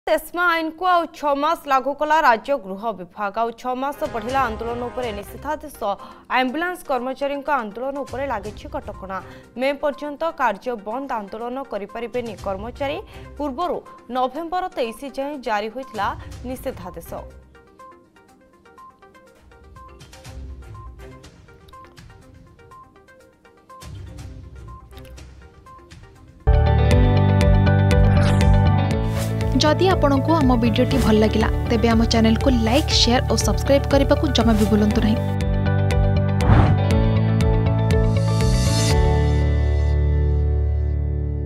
ଏସ୍‌ମା ଆଇନ ଆଉ 6 ମାସ ଲାଗୁ | ESMA Act
ଏସ୍‌ମା ଆଇନକୁ ଆଉ 6 ମାସ ଲାଗୁ କଲା ରାଜ୍ୟ ଗୃହ ବିଭାଗ । ଆଉ 6 ମାସ ବଢ଼ିଲା ଆନ୍ଦୋଳନ ଉପରେ ନିଷେଧାଦେଶ ।